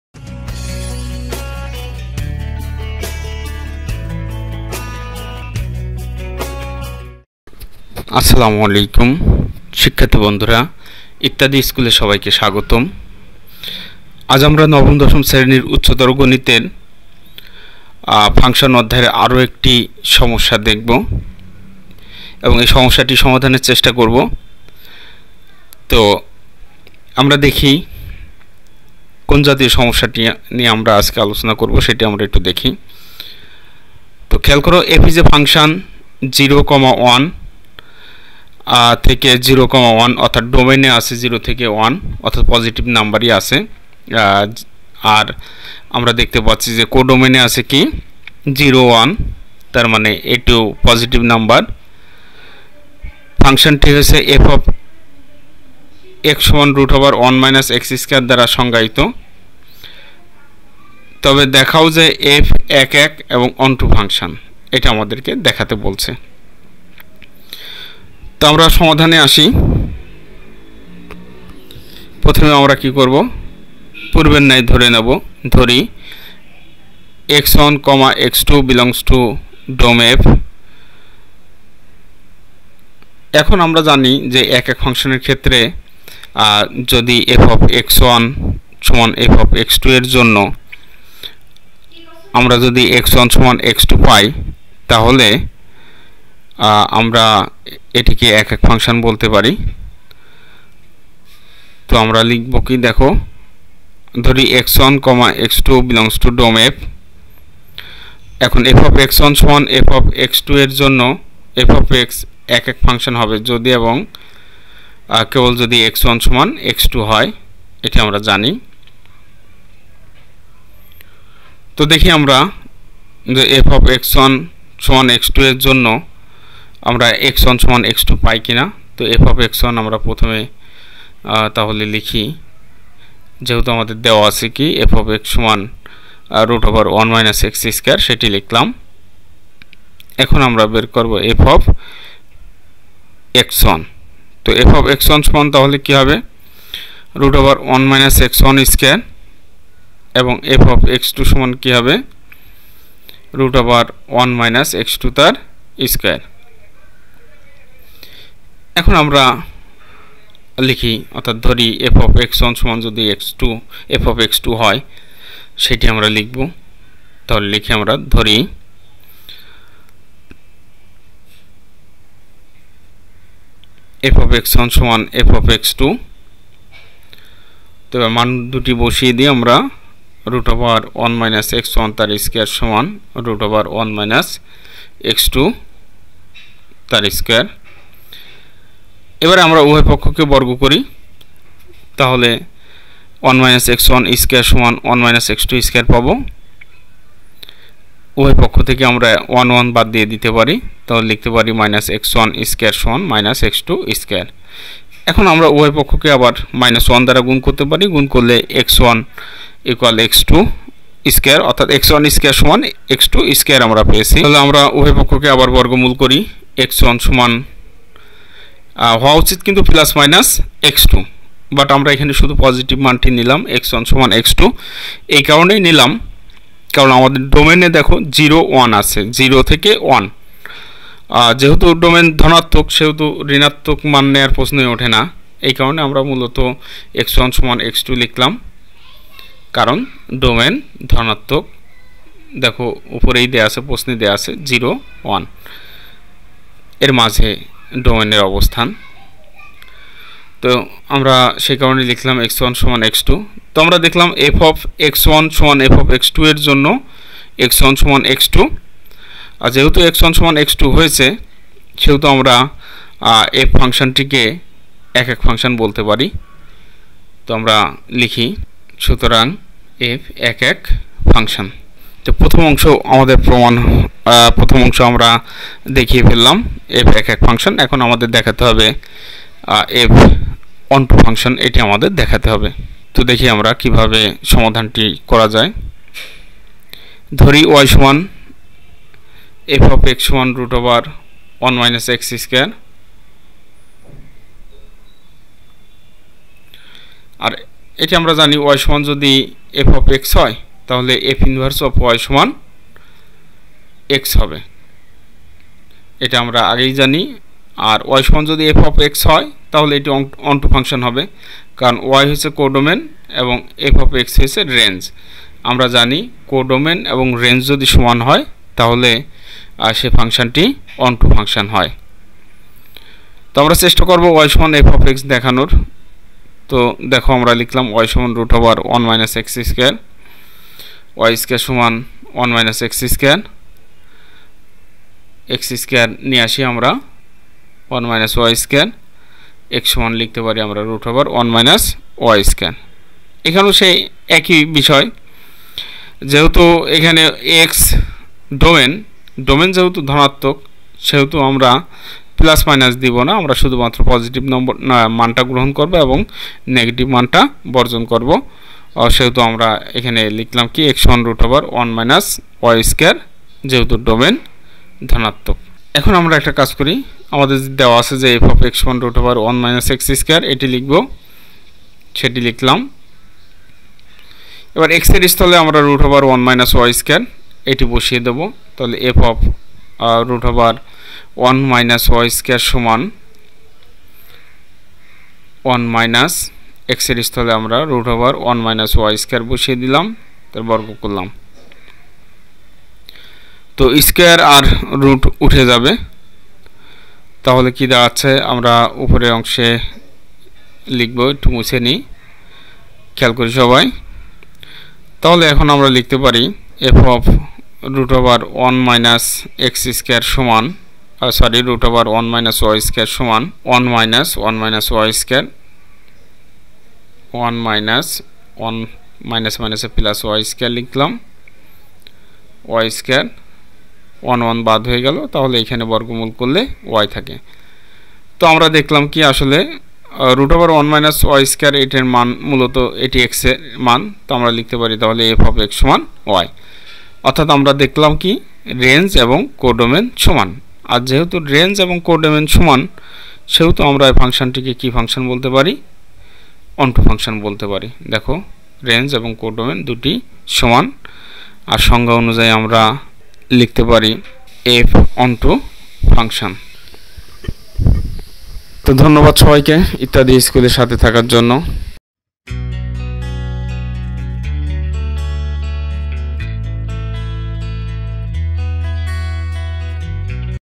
Set and function, ||SSC Higher Math function||ittadischool||(এক -এক ফাংশন, সার্বিক ফাংশন)
# ফাংশন, # সার্বিক ফাংশন
Higher Math Chapter 1.2 (function)  part -2 (One-one function), onto function.
Higher Math Chapter 1.2  (এক -এক ফাংশন, সার্বিক ফাংশন)
SSC Higher Math Function Chapter 1.2. Lecture one-one function.Set and functions.